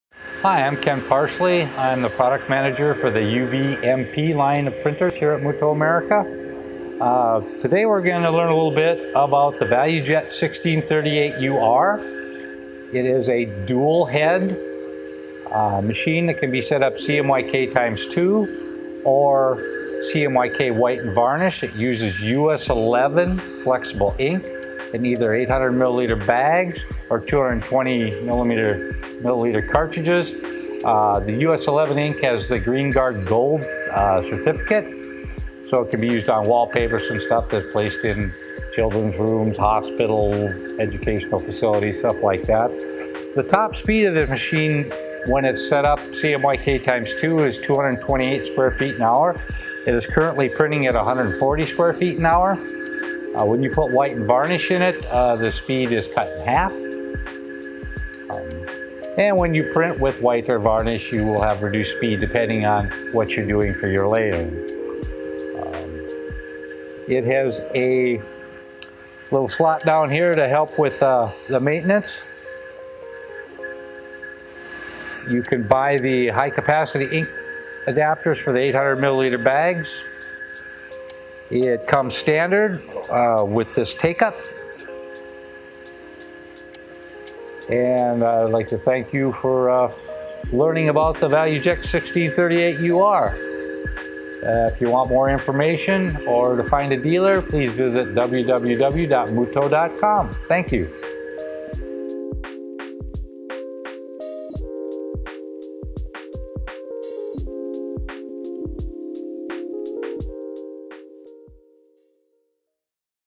ValueJet 1638UR Product Demo
Ken Parsley, Product Manager of Mutoh America's UV/LED and Multi-Purpose line of printers, demo's the ValueJet 1638UR Roll-to-Roll UV/LED Printer.